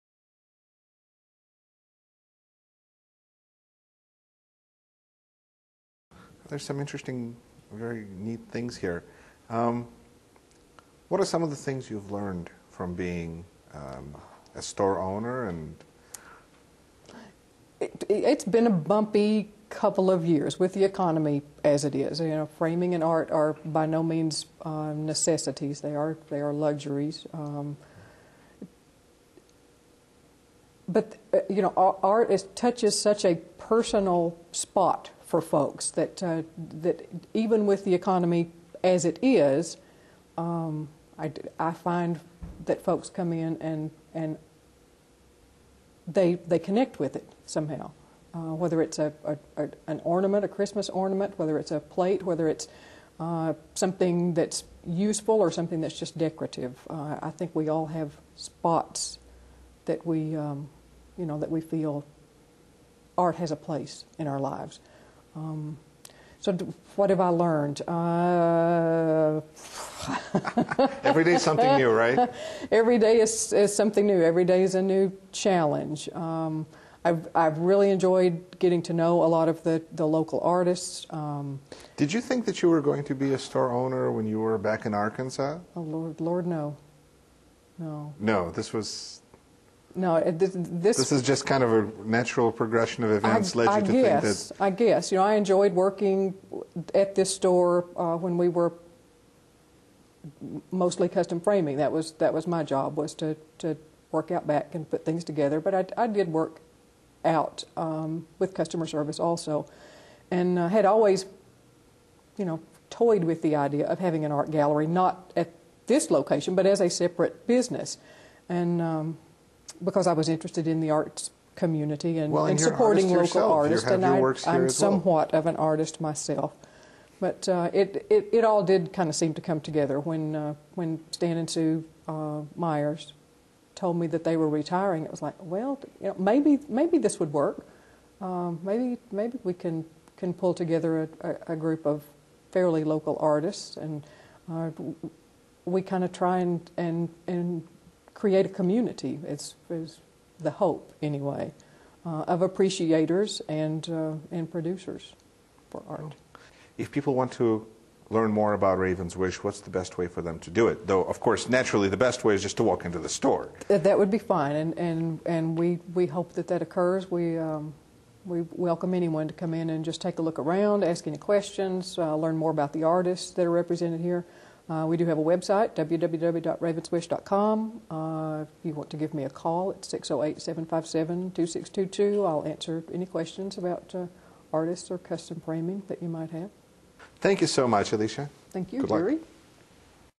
Discover Janesville with Yuri Rashkin - Alicia Reid - part 2
Part 2 of the interview with Alicia Reid, owner of the Raven's Wish in Janesville, Wisconsin.